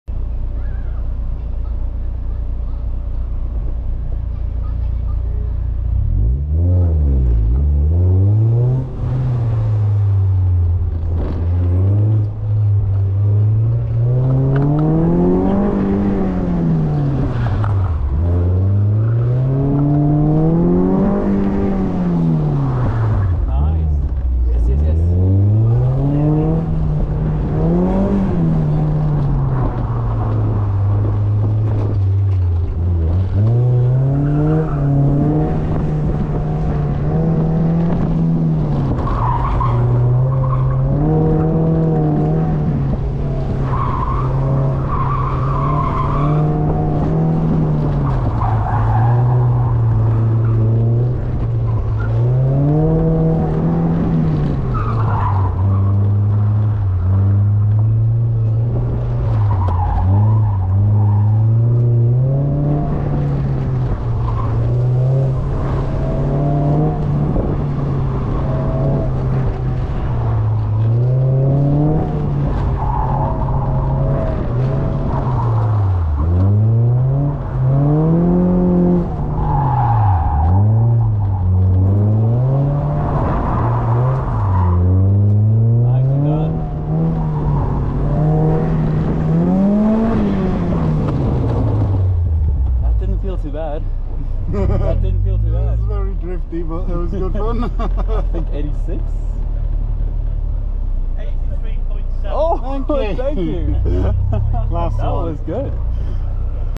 Clwydian Autosolo, Test D, 83.7s
A fun morning skidding about in the rain at Ruthin Farmers Auction.  I got a lot more familiar with using the handbrake although I didn't get it right all the time.  Sometimes I just dragged the back wheels around.